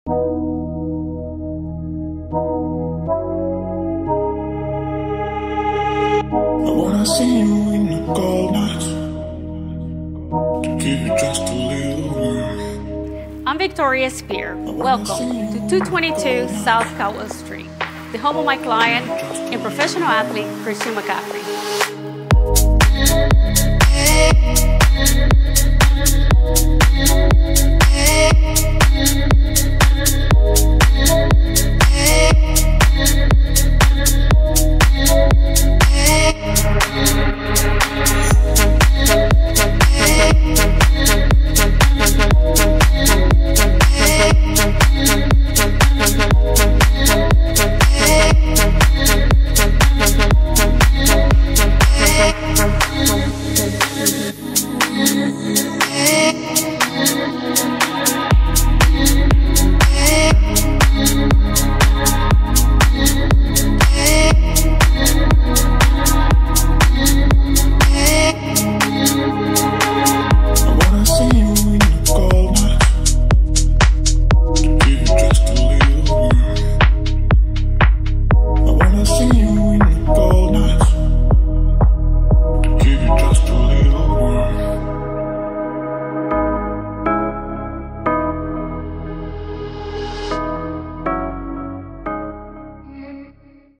Christian McCaffrey's Uptown Penthouse 222 S. Caldwell Street Unit# 2203
CHARLOTTE, N.C. (March 17, 2023) – Over five and a half NFL seasons, Christian McCaffrey set at least 20 team and league records as the star running back for the Carolina Panthers. Now the former Panther, who was traded to the San Francisco 49ers midway through the 2022 season, hopes to set another record with the sale of his Charlotte penthouse.

McCaffrey recently listed the home with an asking price of $3.75 million. If it sells for close to that amount, it will be the most expensive condo ever sold in Charlotte on a per-square-foot basis, according to his real estate agent, Victoria Speer of Ivester Jackson Christie’s International Real Estate.

Located in Uptown Charlotte’s premier Skye Condominiums high-rise building, the 3-bedroom, 3.5-bath, two-story penthouse has 3,813 square-feet of indoor living area plus a large terrace with a 200-squarefoot patio and similarly sized green space. Highlights include a great room with 25’ ceilings; double height windows along two exposures; a chef’s kitchen with stainless steel appliances, cooking island and separate oversized dining island; and a glass and steel architectural staircase leading to the second floor. The main-floor hallway features a barrel ceiling and custom mural by Tampa-based artist, Michael Vahl, which depicts real-life and fictional characters that have inspired McCaffrey throughout his life.

Each of the three bedrooms includes an en-suite bath. Dual owners’ suites – one of the main level and one on the second level – also have large walk-in closets with custom organizers. The marble-clad primary bath has an oversized shower with rain head and a freestanding soaking tub, and the upstairs bath also includes a large shower with rain head and built-in bench, as well as a separate tub. Building residents have access to several luxury amenities, including a roof top pool, fitness center, 24-hour concierge, theatre room, game room, club room, and Fahrenheit, a rooftop fine-dining restaurant from chef Rocco Whalen.

The home is located on the 22nd floor of Skye Condominiums which has a street address of 222. Coincidentally, McCaffrey’s jersey number with the Panthers was 22. In another interesting twist, the buyer pool for McCaffrey’s penthouse may have just gotten a boost after the Panthers recently acquired the #1 overall pick in the upcoming NFL draft from the Chicago Bears. The team’s next star player could very well be the next owner of this residence. “To me, this penthouse was more than a home. It allowed me to feel a part of the city that I love so much every time I look out the window. You can’t find a better layout for a condo anywhere in Charlotte. It’s a home with so much character, and I’m so excited to pass along such an amazing home to the next owner,” said McCaffrey. Added Speer, “The perfect buyer for this penthouse is someone who wants one-of-a-kind luxury, privacy and proximity to all that the city has to offer.” The home is outfitted with furniture by Restoration Hardware, which is included in the list price, along with two deeded parking spaces.